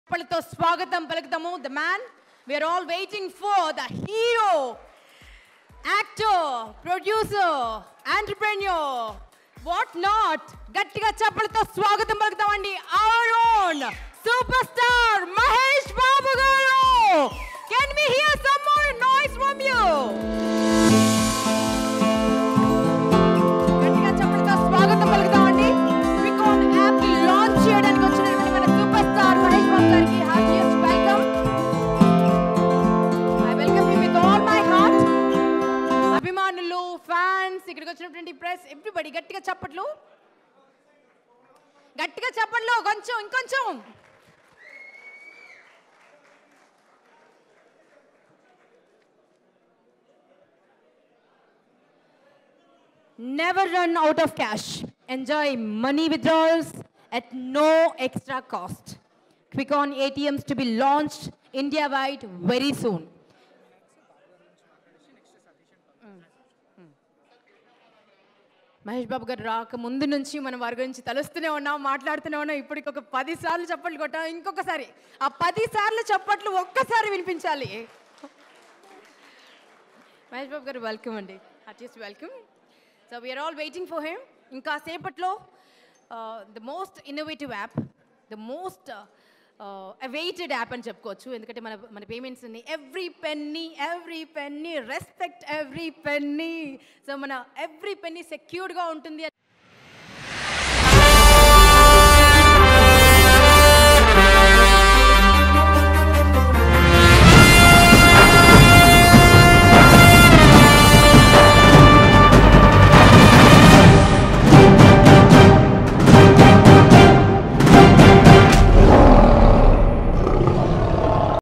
Superstar Mahesh Babu Grand Entry At Quikon App Launch | Leo Entertainment
Leo Politics

Leo Health

Leo Entertainment

Leo Devotional